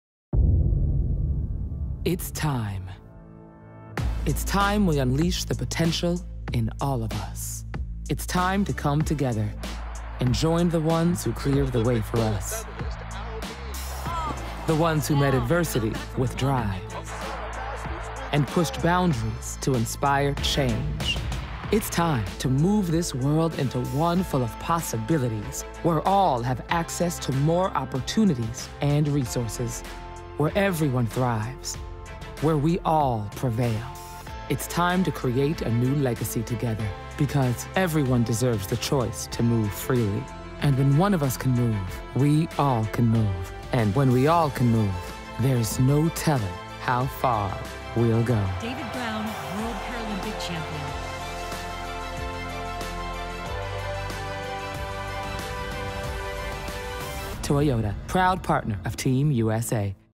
It's Time Toyota Makes Game changing Move to Support the U S
Featuring: Rico Roman, Roderick Townsend-Roberts, Joe Berenyi, Jessica Long, Sam Grewe, Steve Serio, Brian Bell, Al Mead, Oksana Masters, Rob Jones, Lera Doederlein, Alana Nichols, Kaleo Kanahele Maclay, Heather Erickson, Lora Webster, Katie Holloway, Nicky Nieves, Evan Strong, Andrew Kurka, David Brown, Jerome Avery, Amy Purdy, Michael Brannigan, Andre Shelby, and Carlee Hoffman.

It's time we unleash the potential in all of us. Toyota is a proud partner of Team USA and supports the dreams of Paralympic athletes.